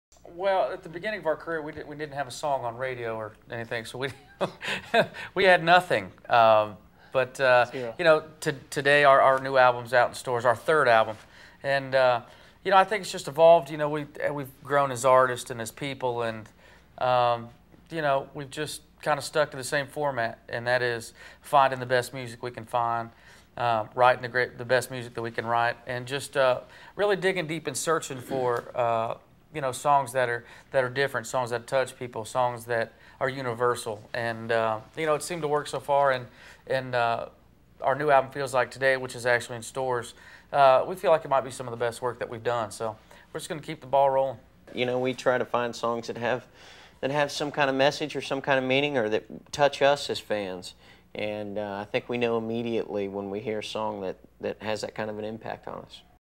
Rascal Flatts Are Ever Evolving!
The gang discusses how they started from meager beginnings and grew into the phenomenon that they are now.